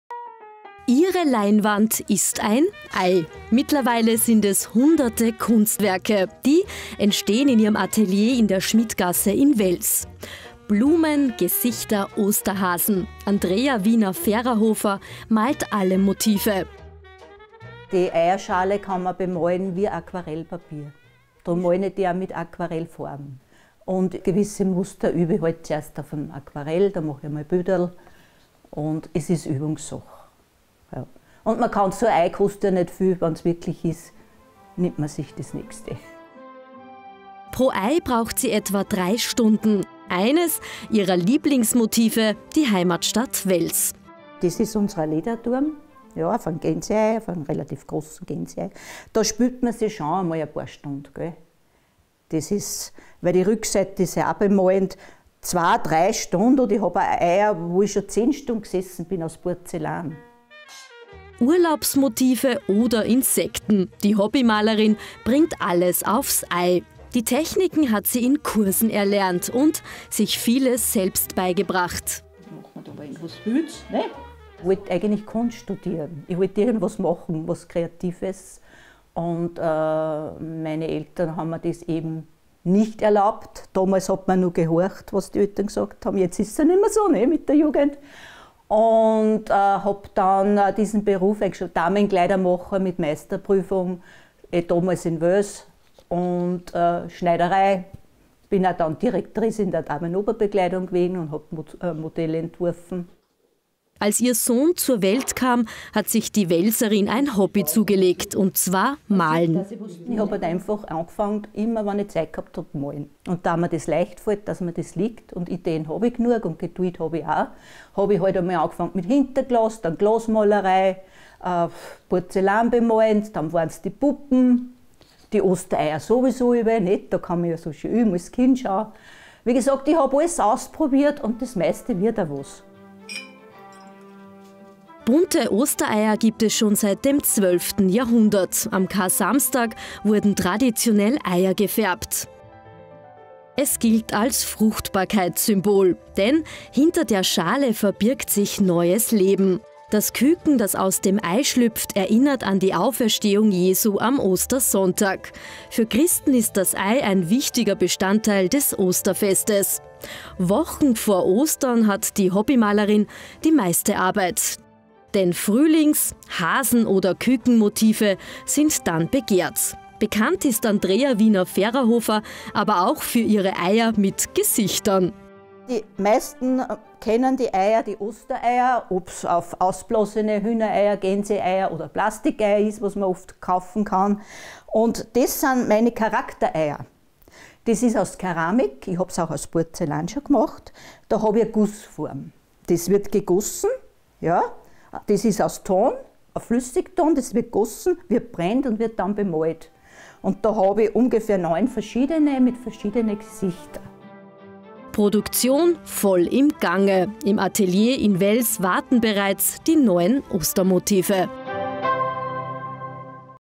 "Fast wie Leinwand": Mini-Kunstwerk am Ei
Ob im Korb, auf dem Strauch oder als Tischdeko. Gefärbte oder bemalte Eier gehören zu Ostern einfach dazu. Das Zuhause mit Osterdeko verschönern, das macht auch Andrea Wiener-Fererhofer. Die Hobbymalerin macht Mini-Kunstwerke. Dafür braucht sie keine Leinwand sondern Eier.